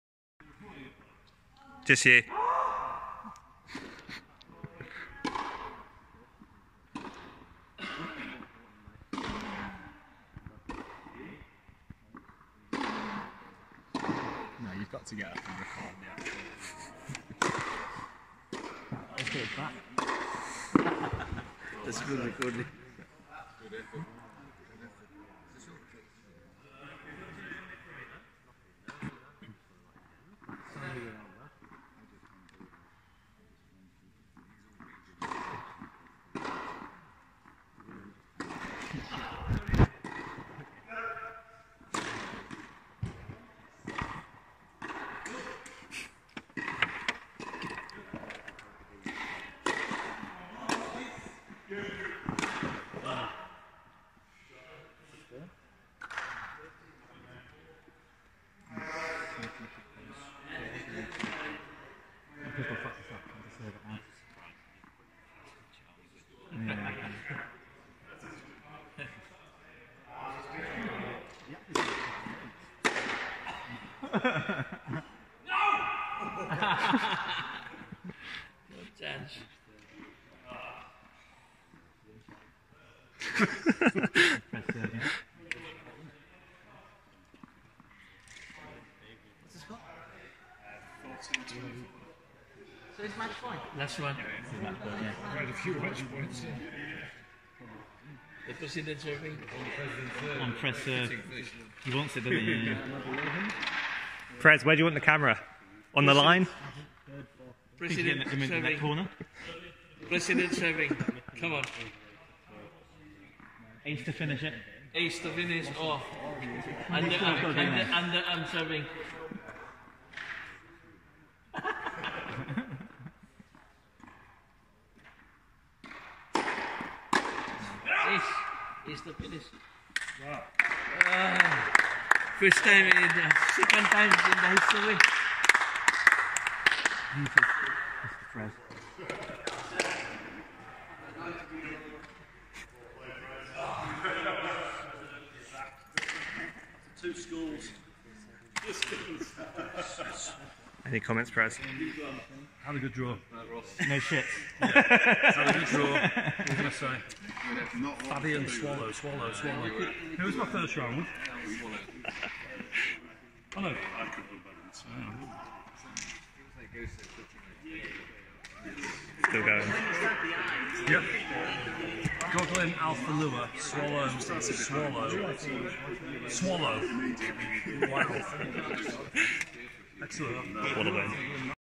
2019 SIC PRO/AM Final Point
The SIC President unleashes hell with a monstrous serve to claim the title